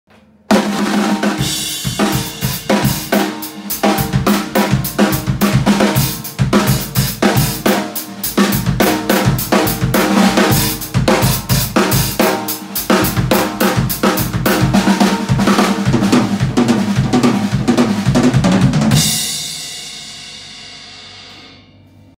Johanna Astrid is 13 years old - testing Drum Limousine drums
13 years old Johanne Astrid (winner of Denmark Got Talent) visited the Drum Limousine Concept Store where she tested our new Drum Limousine Superior Custom drumset.

Sizes:
12"x8" Tom
14"x14" Floortom
16"x16" Floortom
22"x18" Bassdrum
14"x6½" Snaredrum

Wood: American Maple shell with African Marble outerply

Lacquer: Sundown Fade - High Gloss